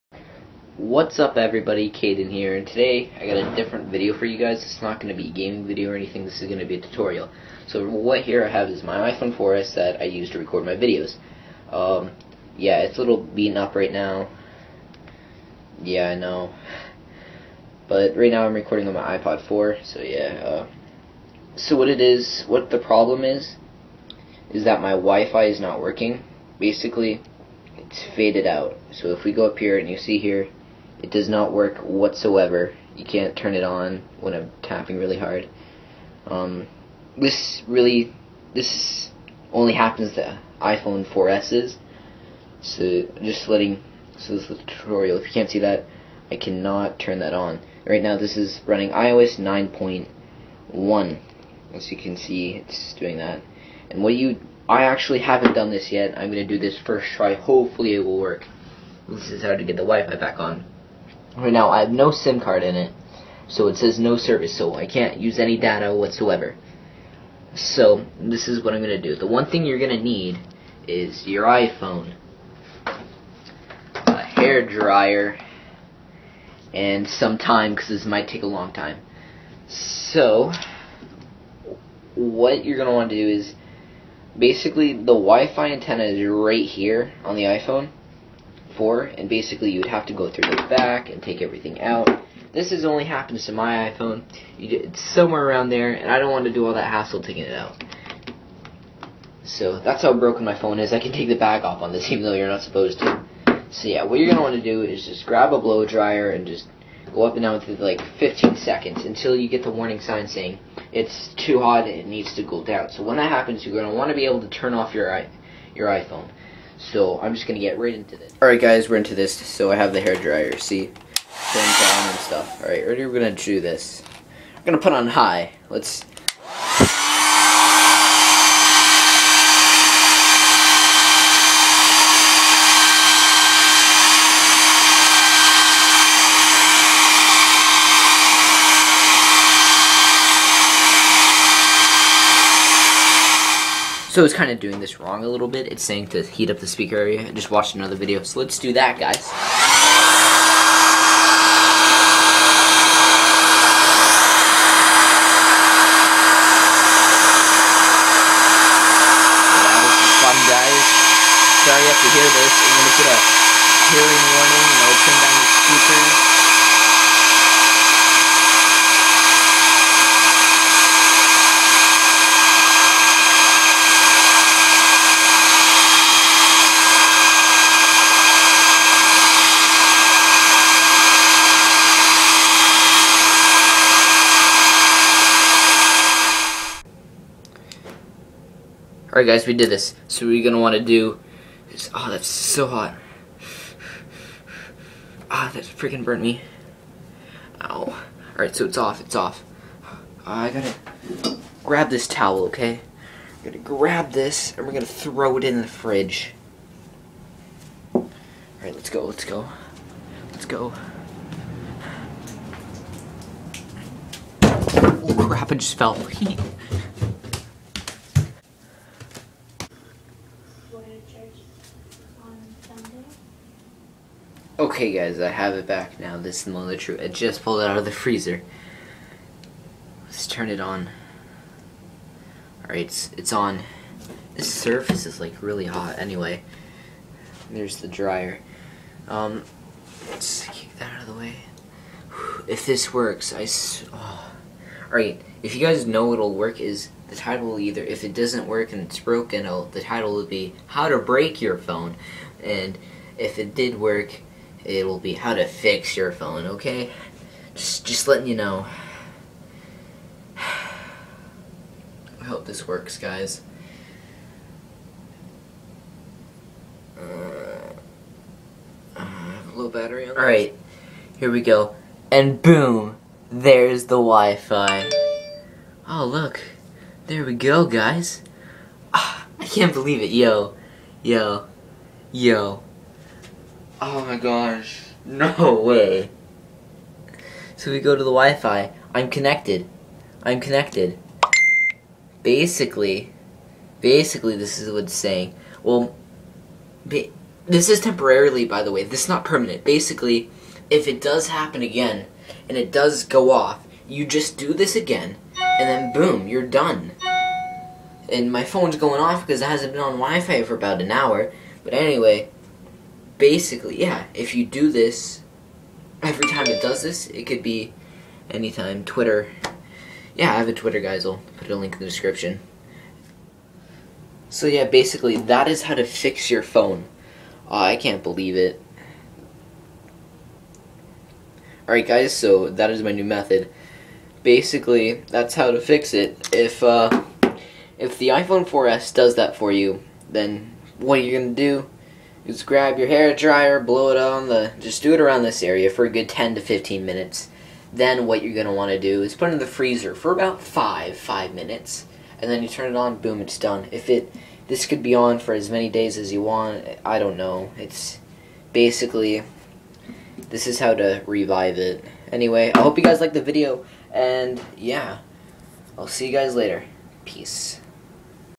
How to fix IPhone 4s WIFI greyed out symbol
Here's how to fix the wifi on the the iPhone 4s running IOS 9.1 and up